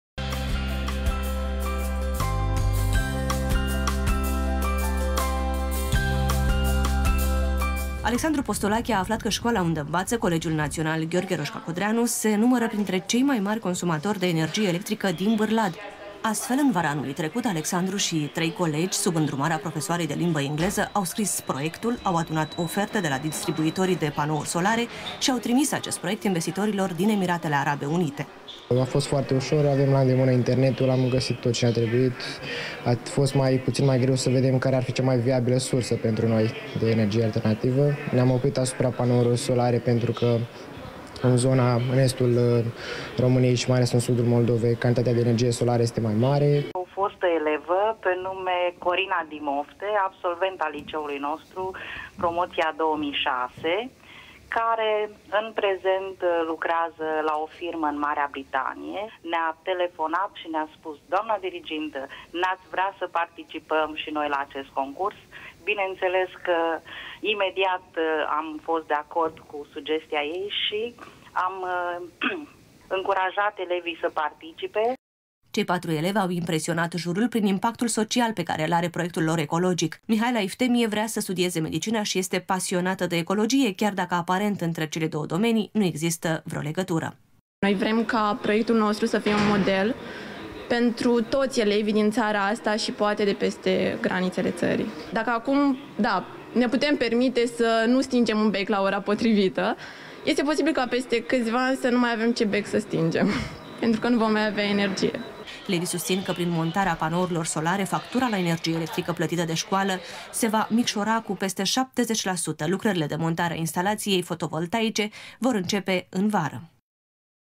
PREMIATI DE EMIRATELE ARABE UNITE
Patru elevi ai Colegiului "Gh. Roşca Codreanu" din Bârlad au câştigat un premiu de o sută de mii de dolari de la Emiratele Arabe Unite. Ei au participat la un concurs internaţional pe teme de ecologie. Banii vor fi investiţi în panouri fotovoltaice pentru şcoală.